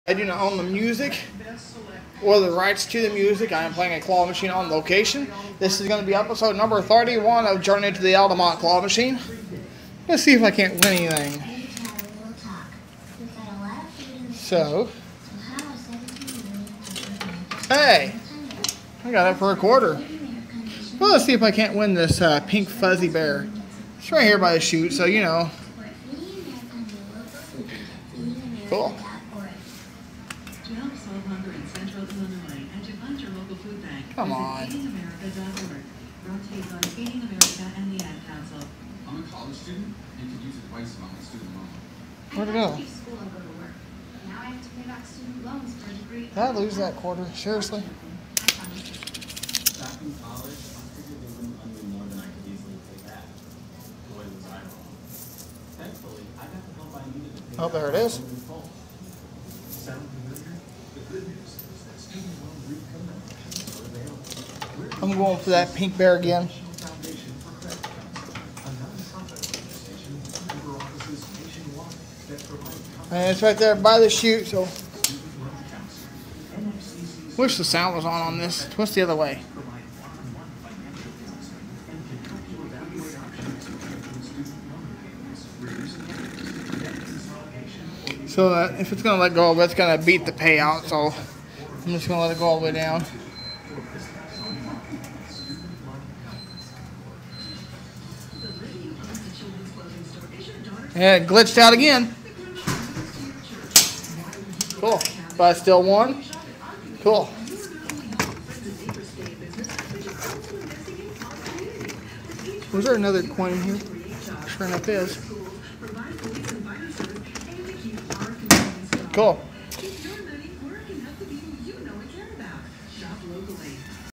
JOURNEY TO THE ALTAMONT CLAW MACHINE EPISODE 31 IT GLITCHED OUT AGAIN
THIS VIDEO WAS FILMED LAST NIGHT AT JOES PIZZA IN ALTAMONT ILLINOIS I RETURN BACK TO THE GLITCHY TOY CHEST AGAIN I DONT KNOW WHY ITS DOIN THAT EITHER HOPE YOU ENJOY THIS VIDEO THANK U FOR WATCHING AND BEIN SO AWESOME HAVE A GREAT DAY